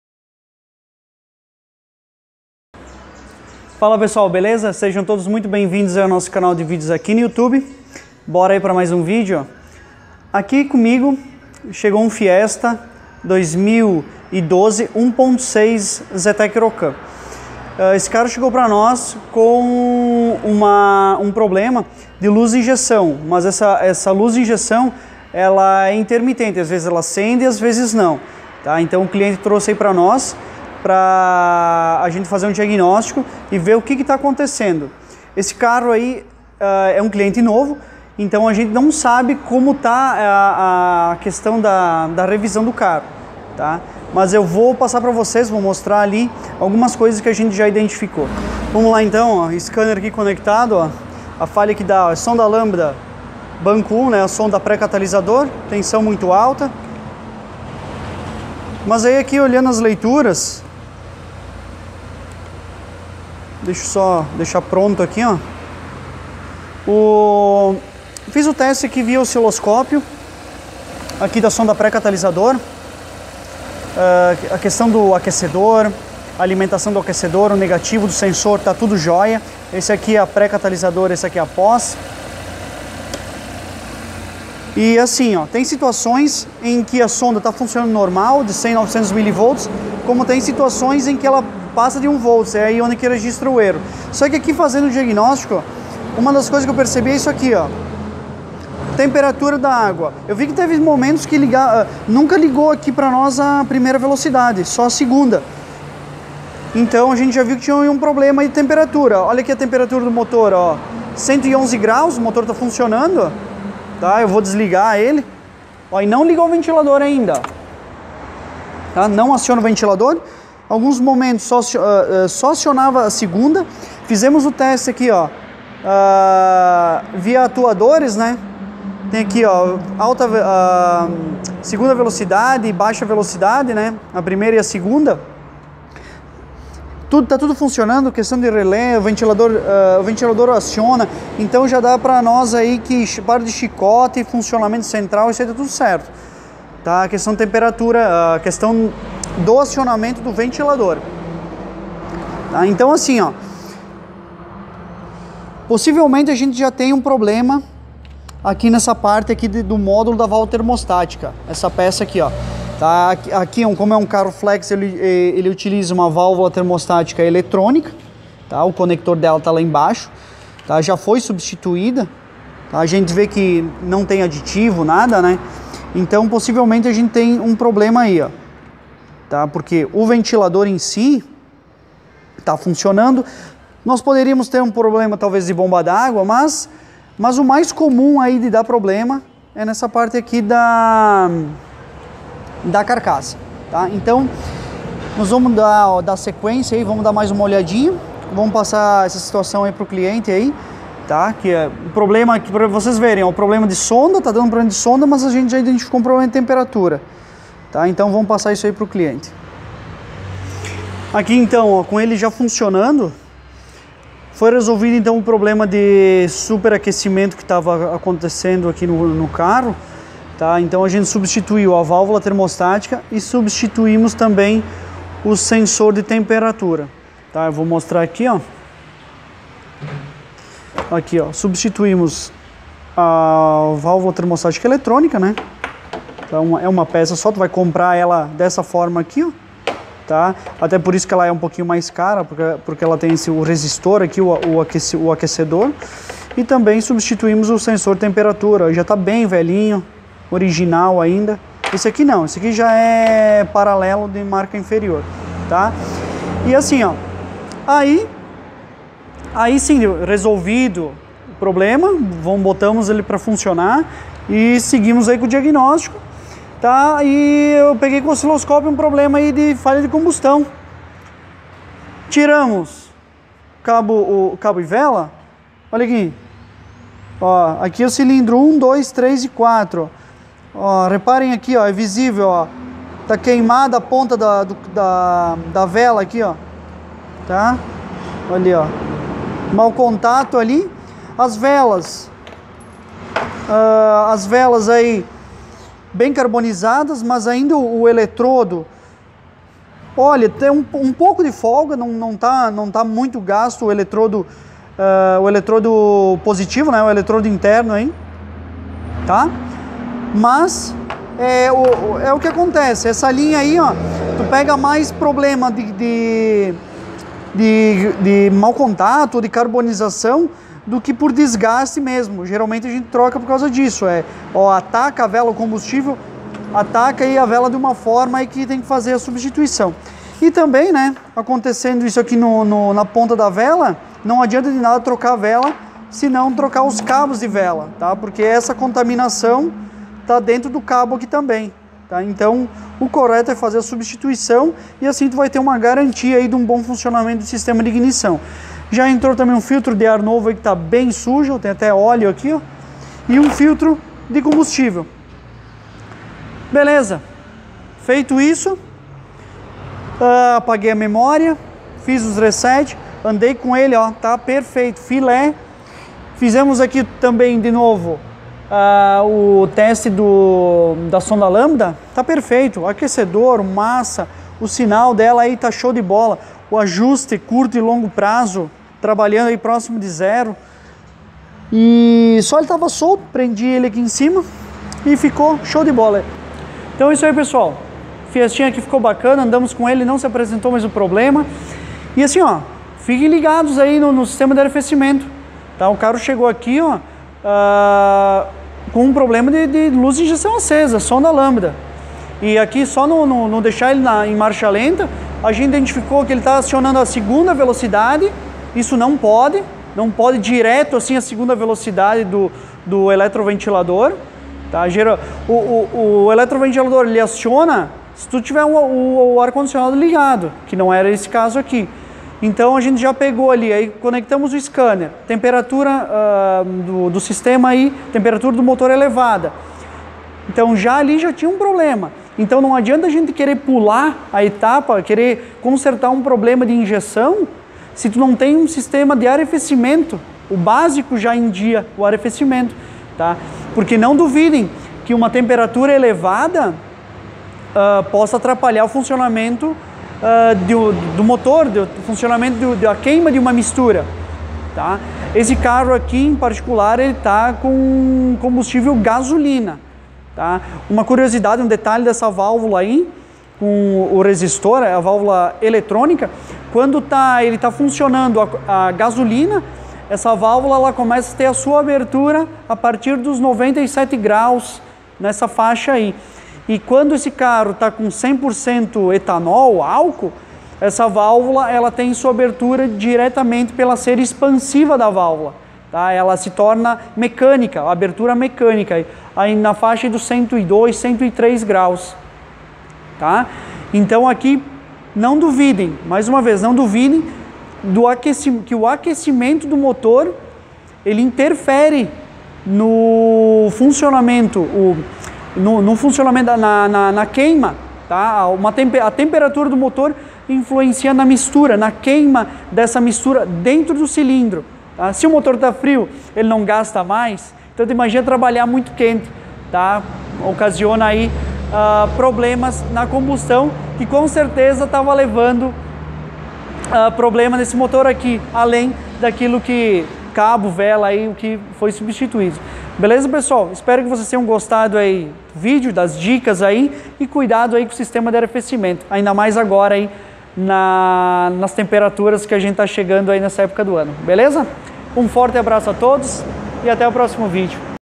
FIESTA ZETEC FLEX - Luz da injeção acesa e perda de potência
MECÂNICA PAGLIARINI

RS 129 Km 73 N°6785  Bairro Lambari

Encantado Rio Grande do Sul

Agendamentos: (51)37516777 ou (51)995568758